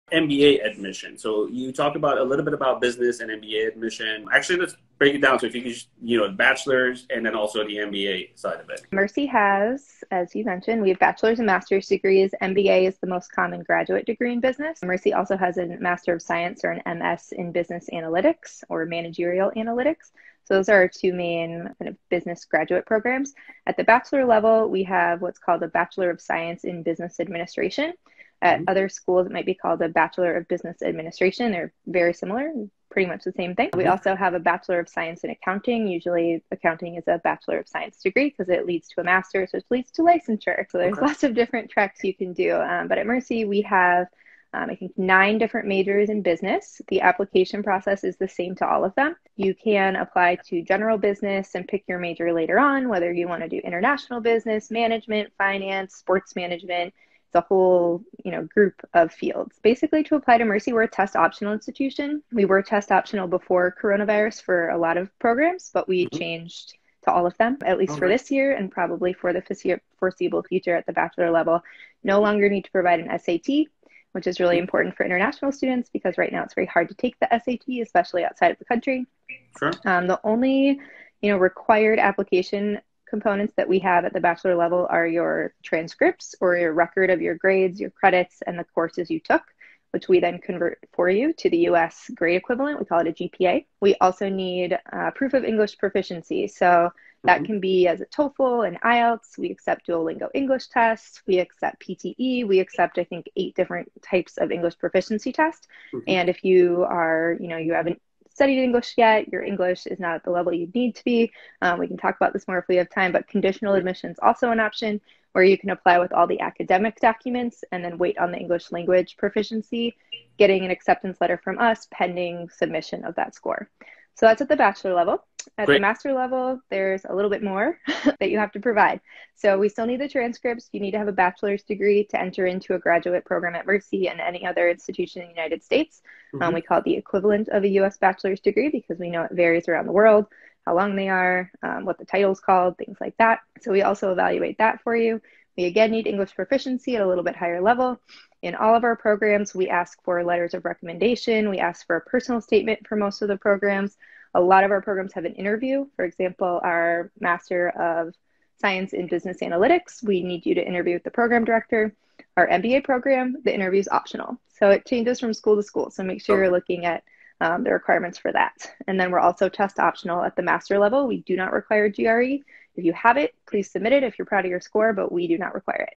Mercy College İşletme alanında lisans ve lisansüstü programlar
Mercy College İşletme lisans ve lisansüstü programlar
Daha fazlasını öğrenmek için Ashkan ve Sonya Brauchle'nin görüşmesine katılın🎙️